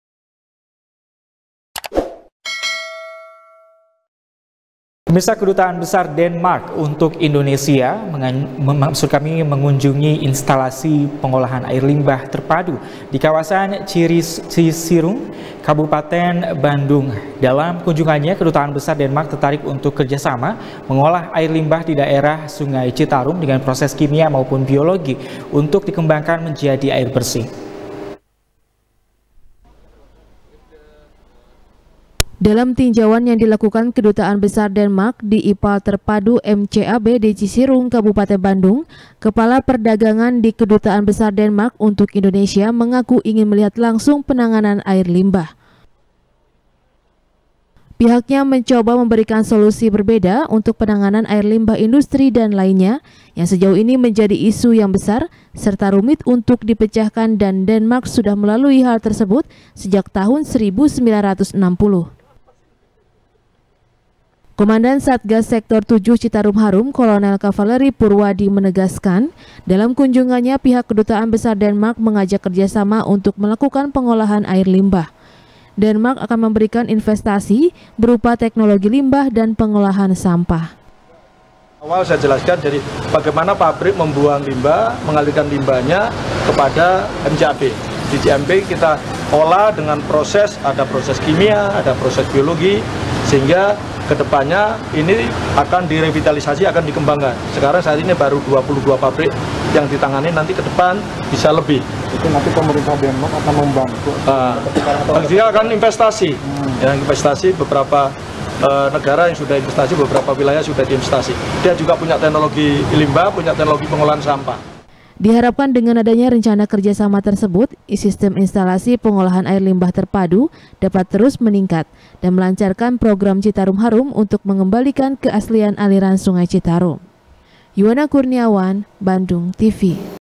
Kedutaan Denmark Ajak Kerjasama Tuntaskan Limbah Citarum | SBR | BANDUNGTV
SBR | BANDUNGTV | IRFAN RAMANDA | Kedutaan Besar Denmark untuk Indonesia mengunjungi Instalasi Pengolahan Air Limbah Terpadu, di Kawasan Cisirung Kabupaten Bandung.